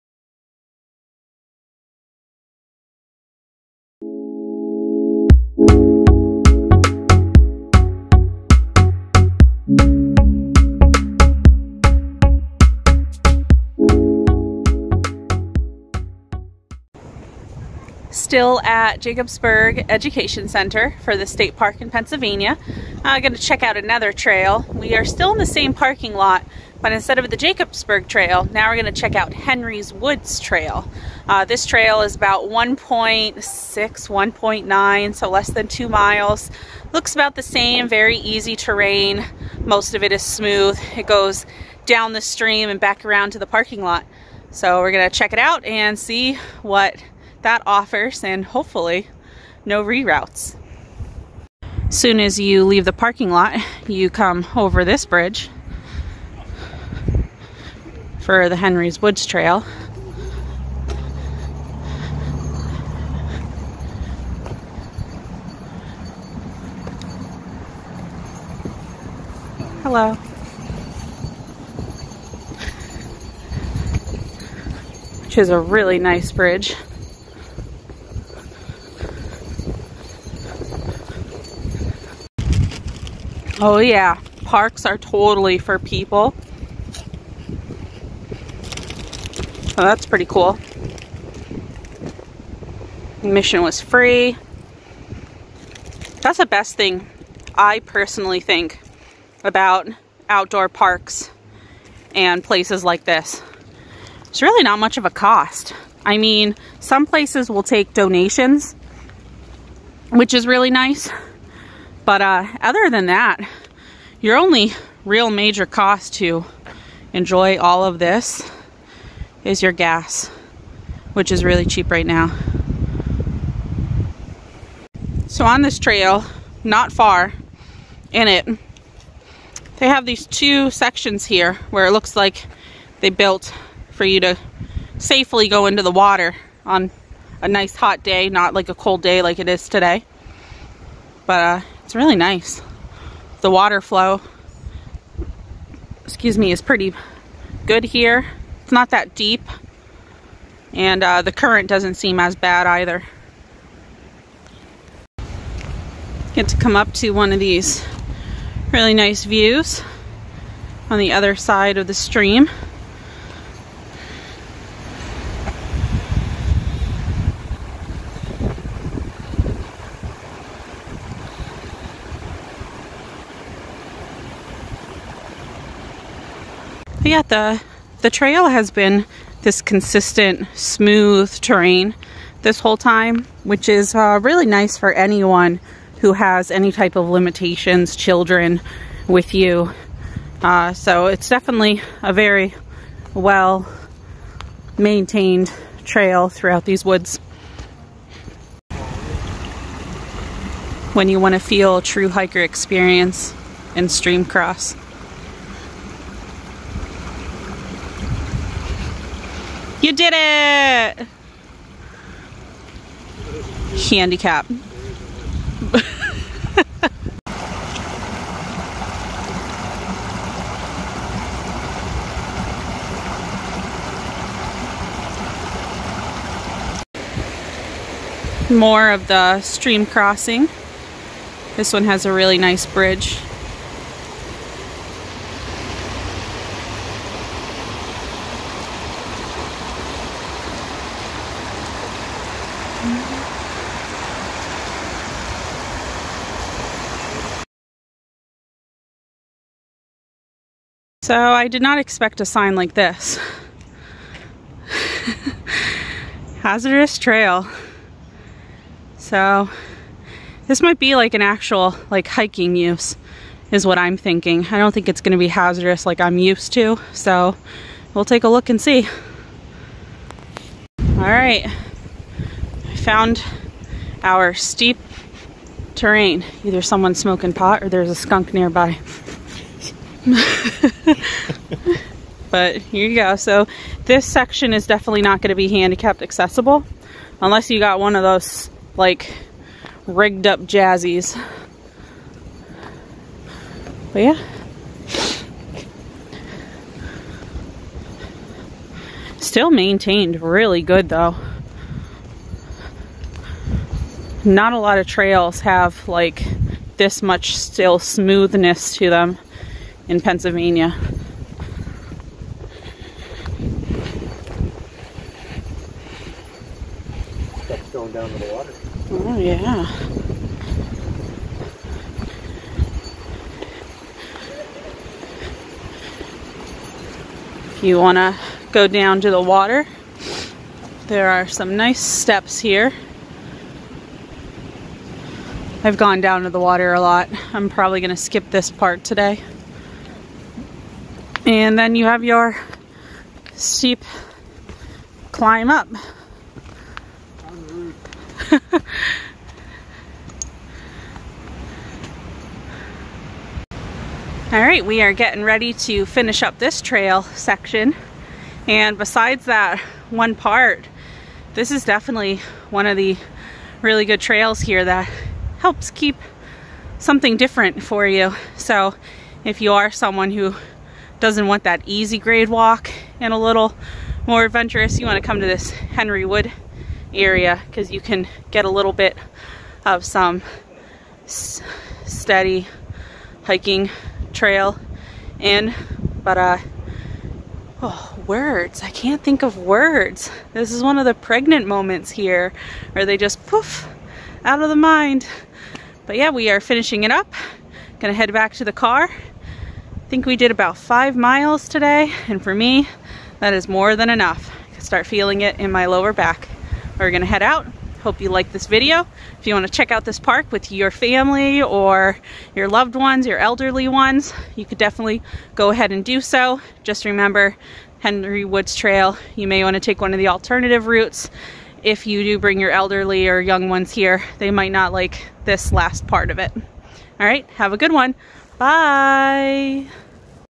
Henry Woods Trail
Part 2 to the hiking at Jacobsburgs Educational Center. This trail started off smooth and then went to steep, which was unexpected. Not handicapped accessible on the difficult side we took. There is another trail that loops back to the parking lot with this one that can make it handicapped but would add more miles. This center has many trails and nature scenes to check out. A must go!